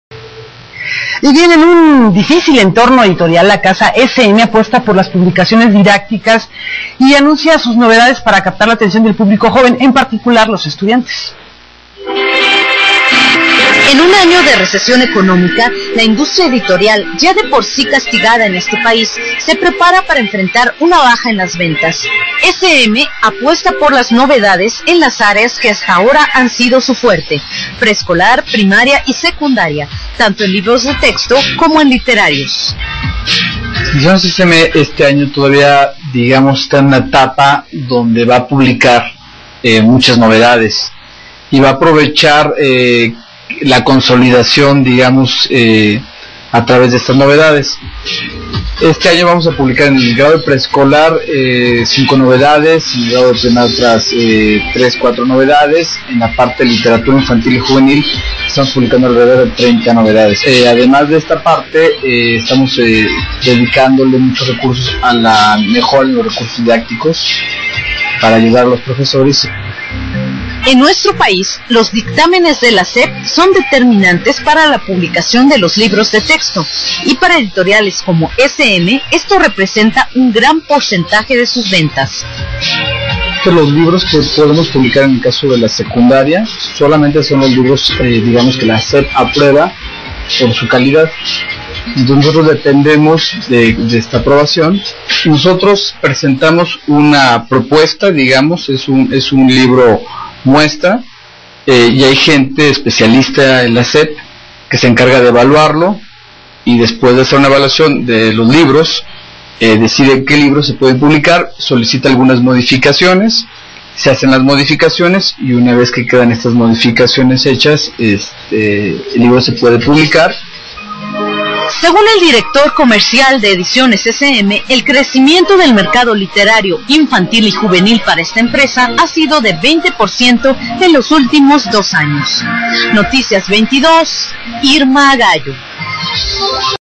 noticias22
Nota transmitida por el canal 22 sobre Ediciones SM